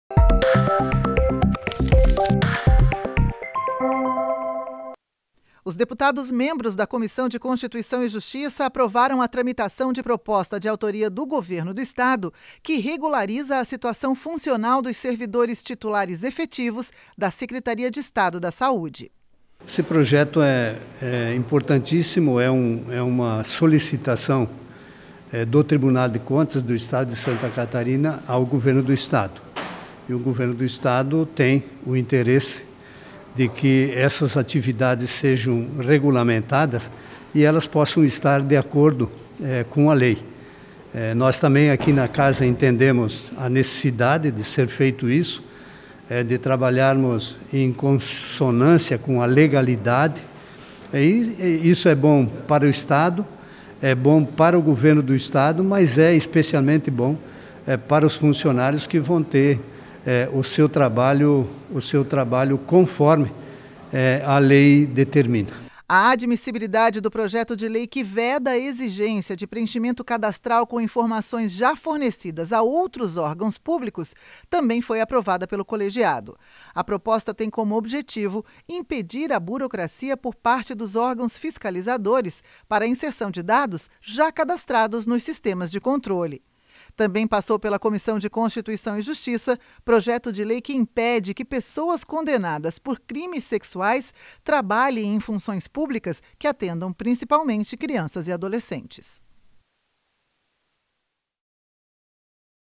É Notícia - CCJ admite proposta que regulariza situação de servidores da saúde - 21/09/21
A Comissão de Constituição e Justiça aprovou proposta de autoria do Governador do Estado que trata da situação funcional dos servidores titulares de cargos de provimento efetivo da Secretaria de Estado da Saúde. Além disso mais dois projetos foram admitidos pela comissão, um deles tem como objetivo diminuir a burocracia na inserção de dados em preenchimento cadastral com informações já fornecidas a outros órgãos públicos, o outro impede que pessoas condenadas por crimes sexuais trabalhem em funções públicas que atendam principalmente, crianças e adolescentes.